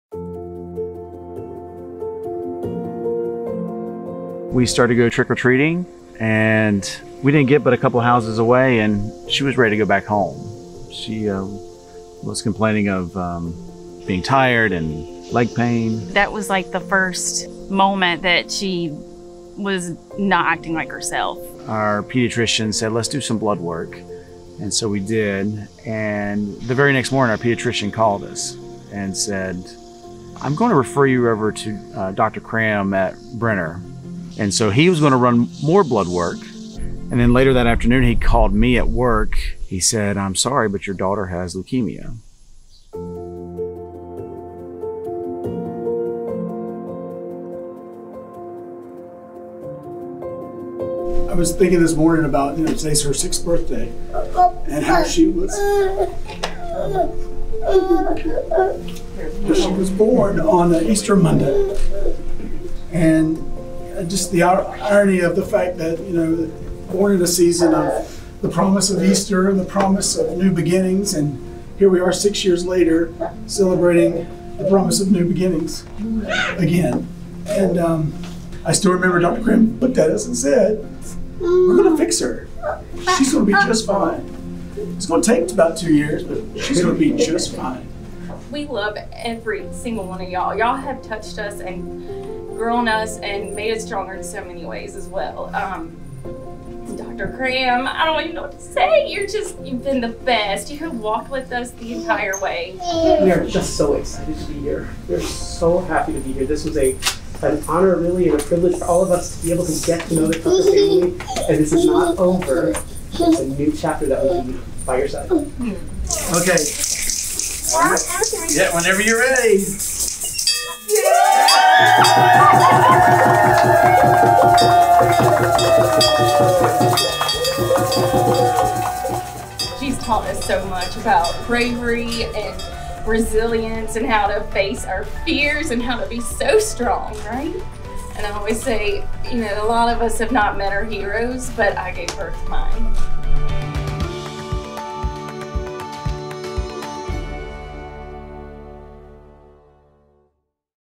Roe Roe's Fight | Cheers! for Brenner Children's
Facing a family cancer diagnosis is hard. Being the parents of a child with cancer may be one of the hardest challenges a person can face.

That is the challenge Fox8 news anchor Chad Tucker and his wife, Meredith, had to face when their daughter Pearl Monroe Tucker – "Roe Roe” to family members – when she was diagnosed with leukemia.

March 28, 2021 – on her sixth birthday and two years after her diagnosis – Roe Roe rang the bell at Atrium Health Wake Forest Baptist's Comprehensive Cancer Center to celebrate the end of her cancer treatment and the start of her next steps toward healing and recovery.

A child’s cancer diagnosis changes many things and, for many families, financial support can make a significant impact during the journey

On Nov. 4, we're celebrating patients like Roe Roe and helping make dreams come true ForAll patients during Cheers! for Brenner Children's. Purchase your ticket today